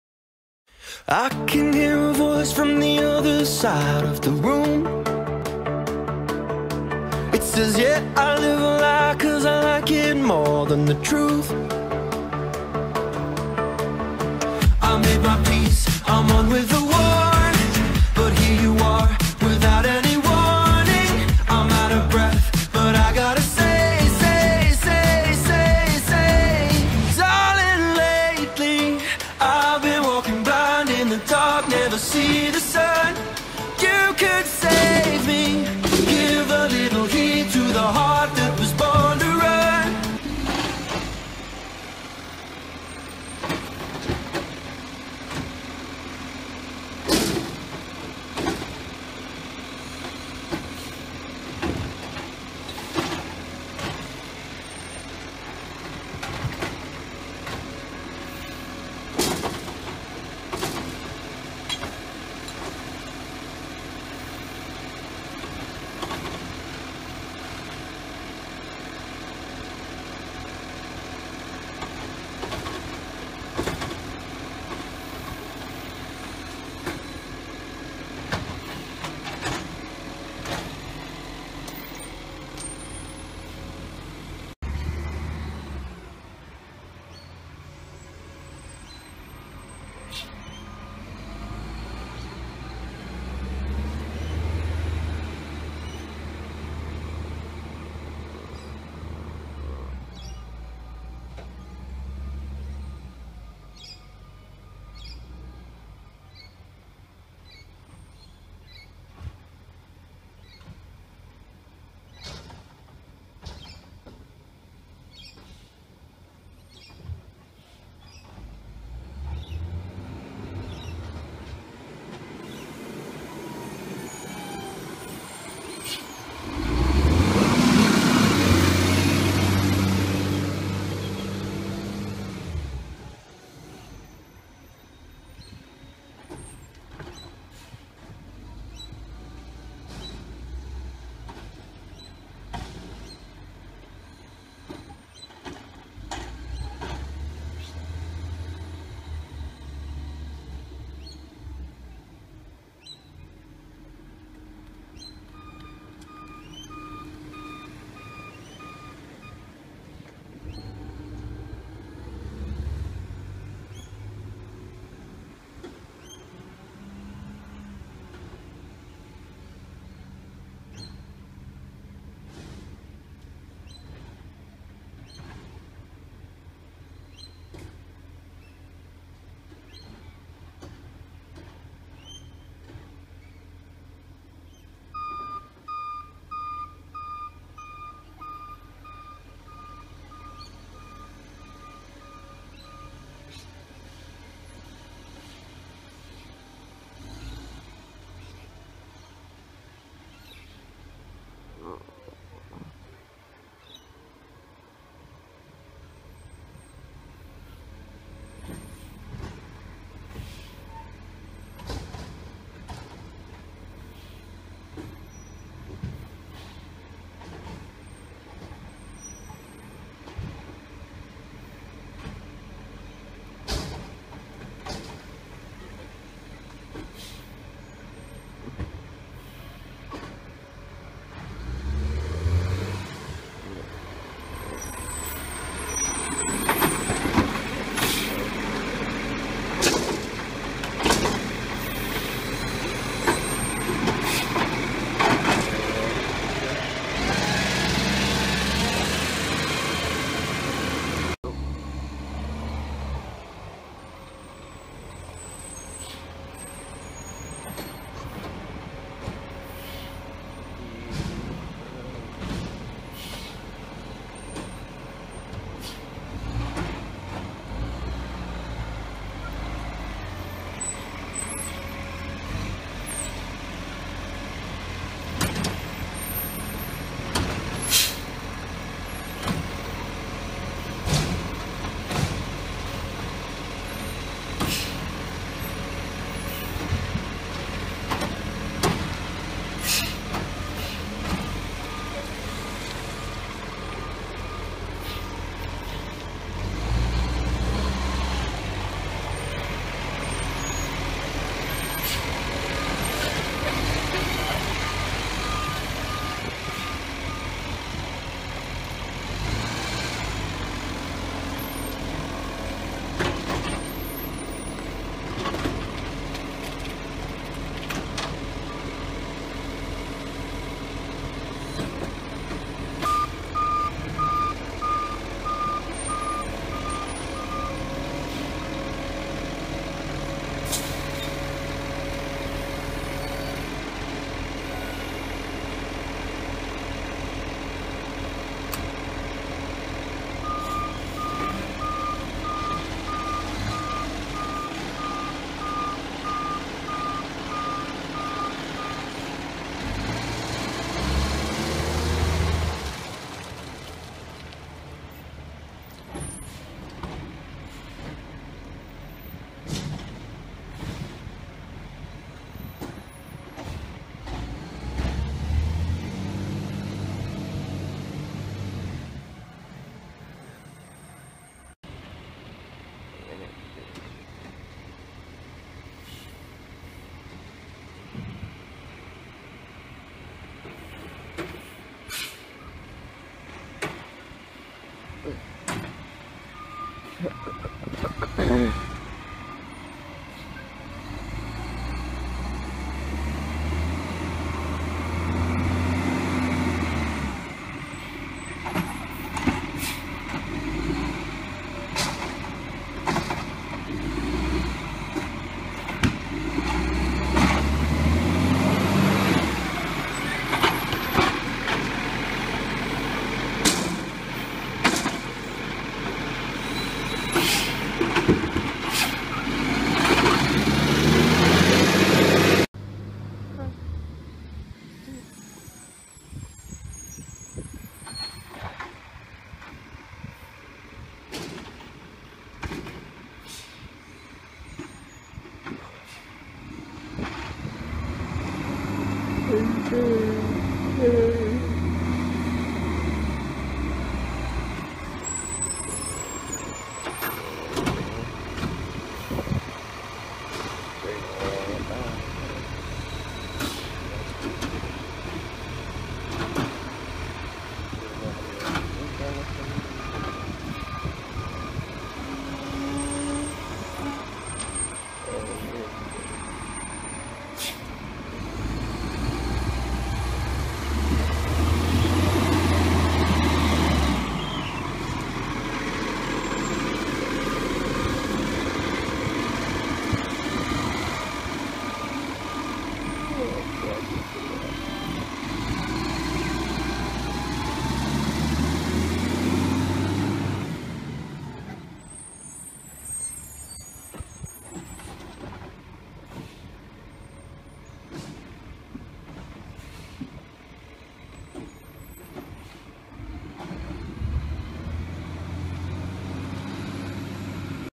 Wyong Greens, Truck #67, mates footage
One day, my best mate and I decided to film the normal trucks I would film in this route. I got him to film with my old cam and he did a really good job i thought.

Anyway back too the main point, you know when you got a good mate when he purposely wants too come and video with you, even though he doesn't really have a interest in the industry.

As always thanks to the driver for letting us film.